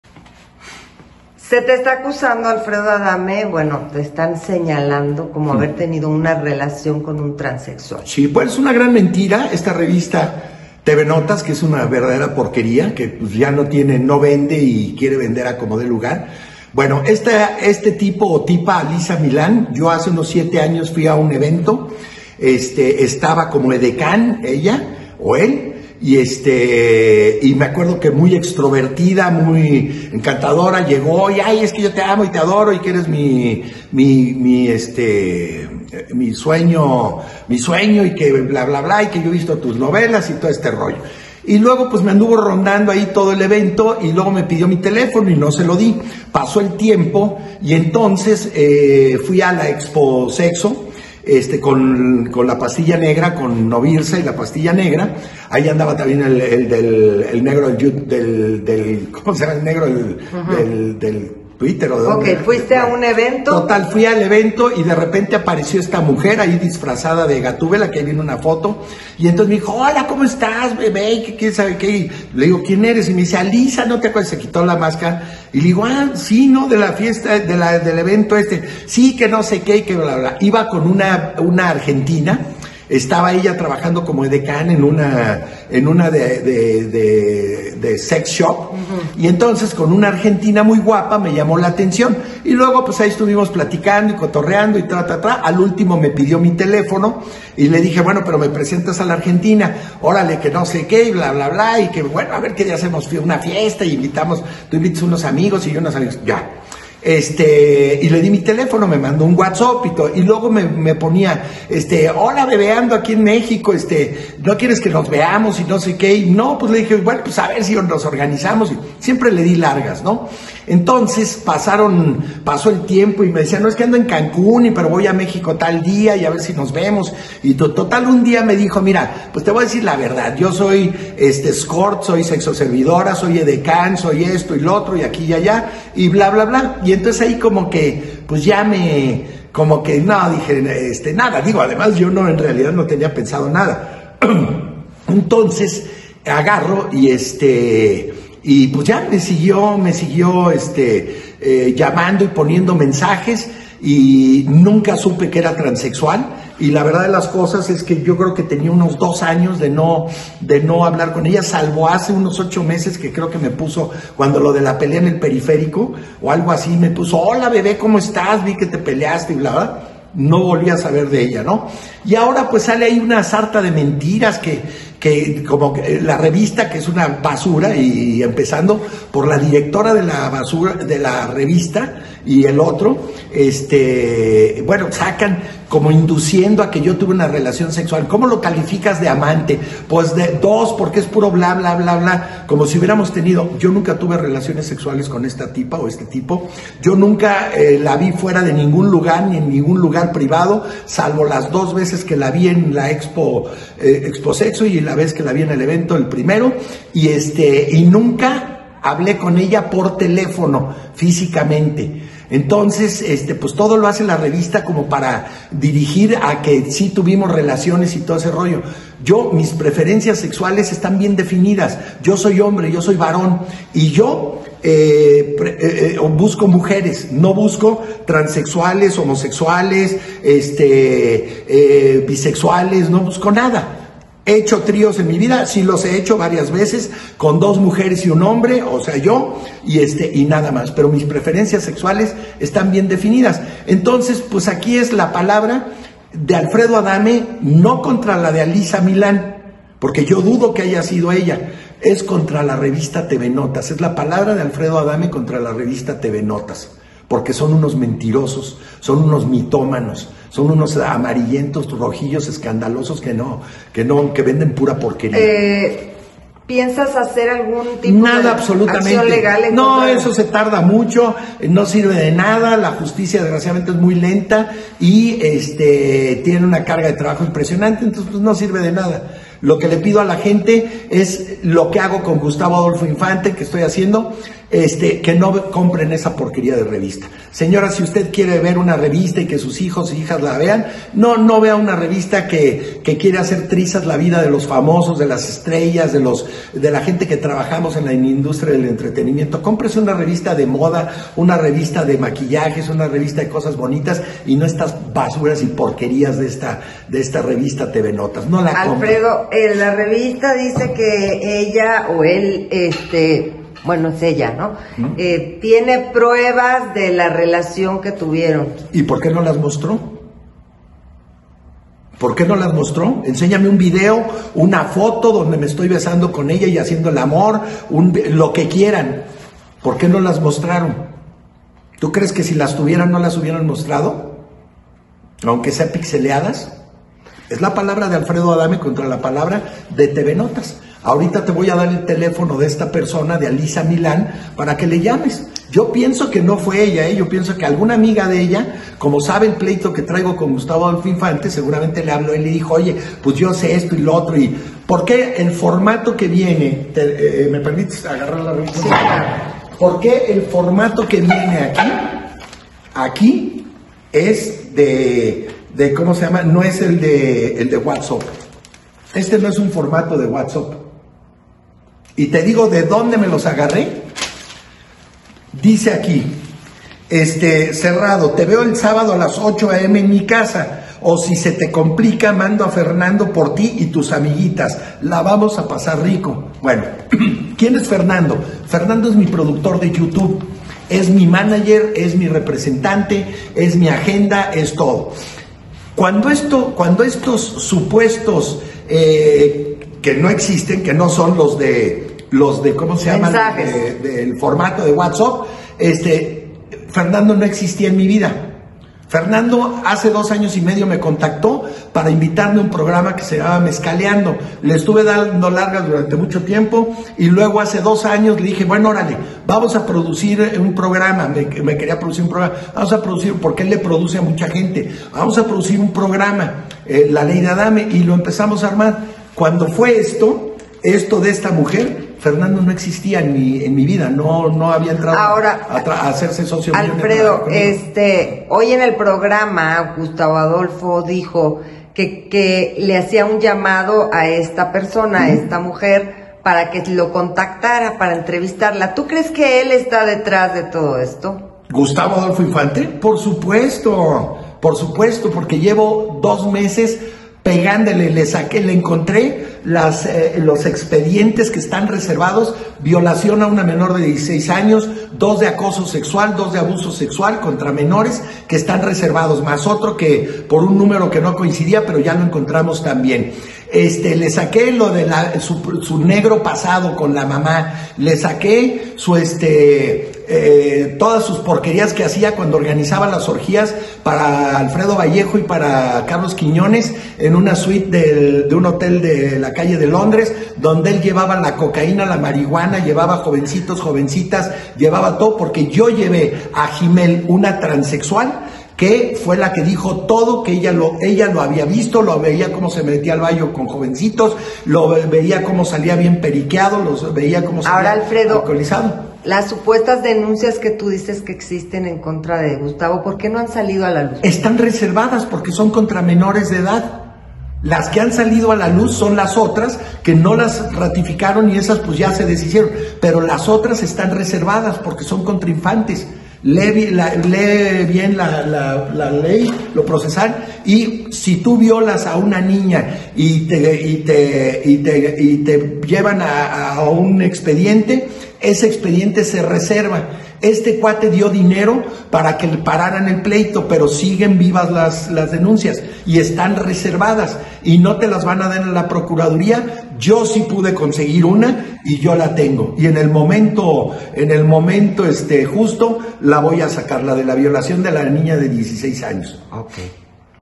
ADAME ACLARA RELACION CON TRANS
Adame nos cuenta todo sobre la relación con la mujer trans